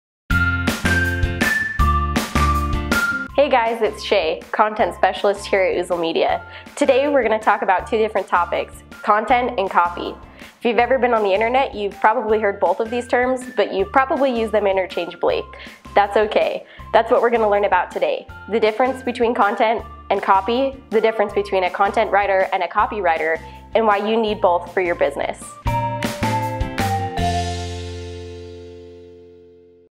Online Content Marketing Strategy 2017 - Content VS Copy
Online Content Marketing Strategy 2017. What is the difference between Content and copy? Copy and content: both of these ideas are important to websites, ads, landing pages, and blog posts. You may have heard these terms being used in reference to internet marketing or marketing in general. If you’re a business owner, you’ve probably heard them from your marketing team or agency. While the two terms may be used interchangeably, they are not the same thing. What you need to know:
- The difference between copy and content.
- What makes content and copy good.
- The difference between a content and copywriter.

Come visit our Utah online marketing office to learn more information.

We’re always happy to help!